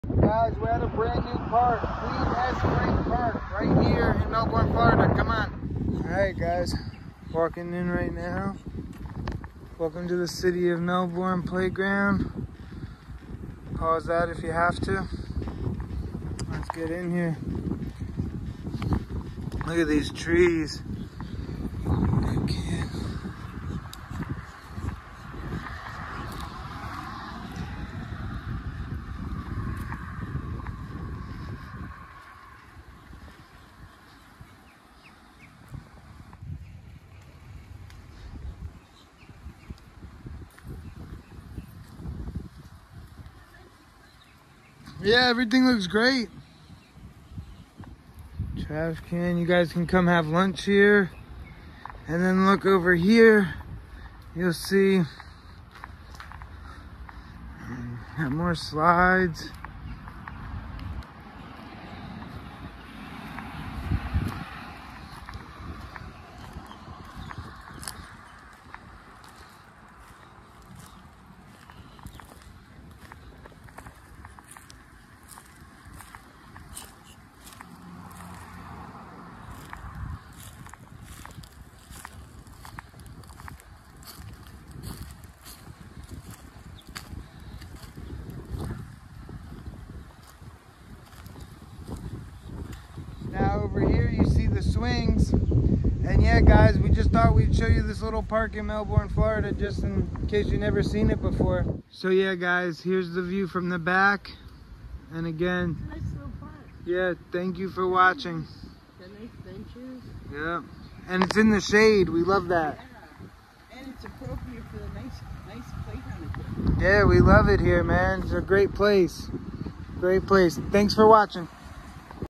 Exploring Cleave S. Frink Park in Melbourne Florida
Cleave Frink, the longtime activist who rallied against drugs in Melbourne, pushed for bettering police relations, and who helped desegregate lunch counters in dime-store dinettes during the civil rights movement, has died. He was 78.
Frink, who was also a former president of the South Brevard NAACP and a minister of Greater Life Fellowship Ministries, died Tuesday after a short illness, family members said.